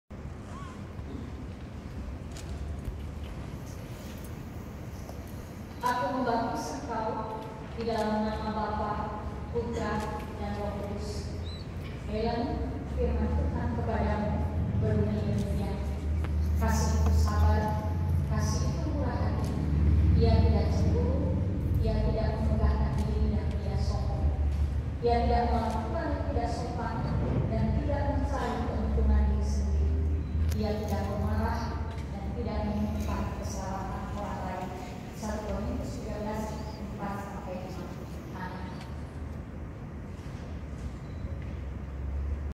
Eileen Adelina Chandra
Eileen Adelina Chandra @eileenchndr di SAKRAMEN BAPTISAN KUDUS DEWASA dan PENGAKUAN PERCAYA (SIDI) di GKI Maleo Raya di Kebaktian Umum Pkl. 06.00 wib tgl. 17 November 2019 yg dipinpim oleh Pdt. Des Elbiat Gulo.